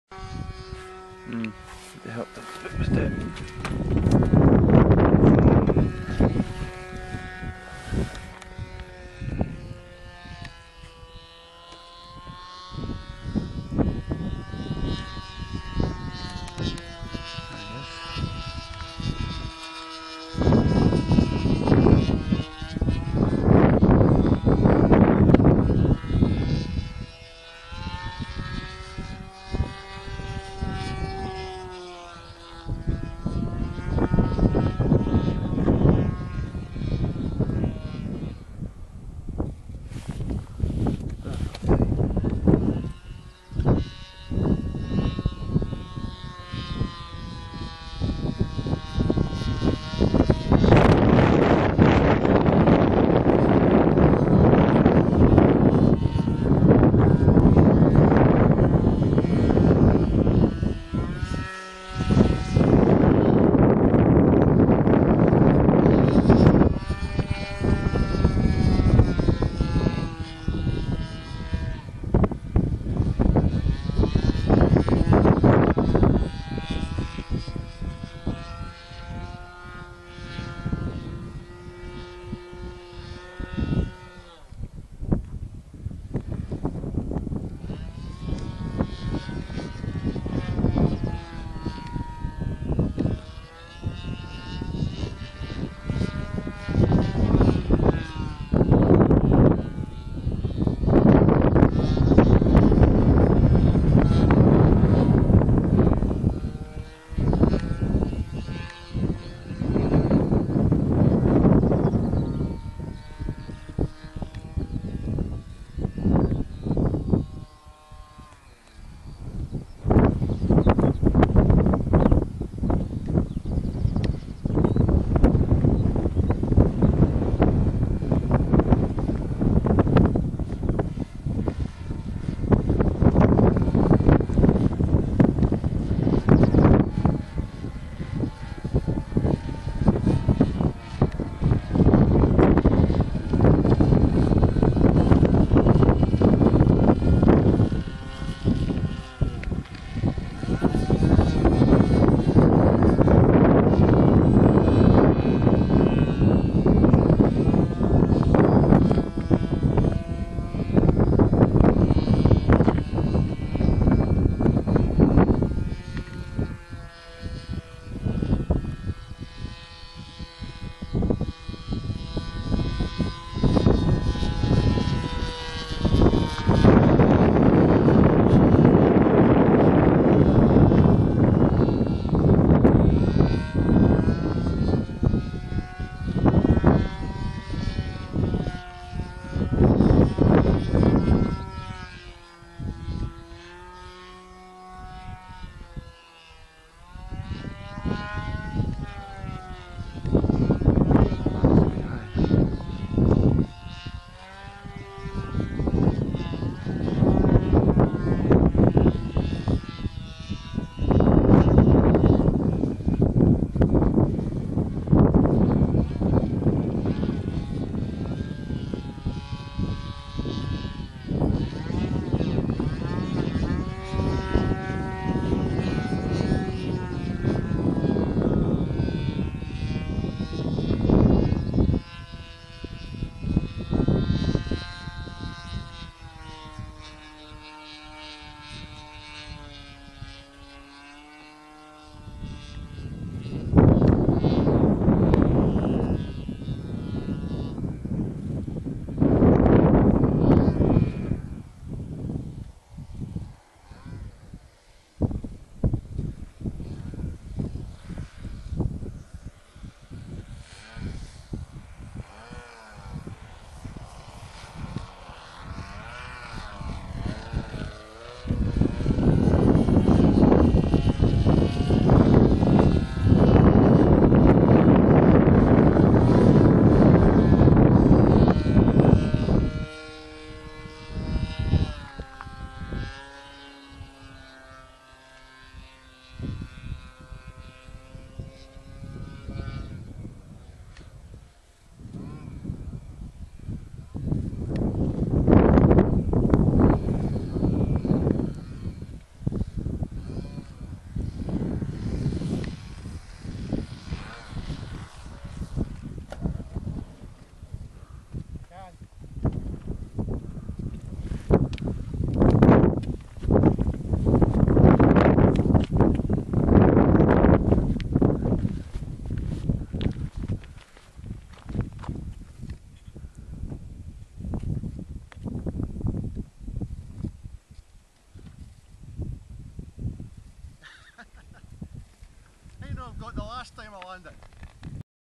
blackhorse speed air mild aerobatics
jim flying his blackhorse speed air bought from kingslynn models
irvine 46 engine 11/6 prop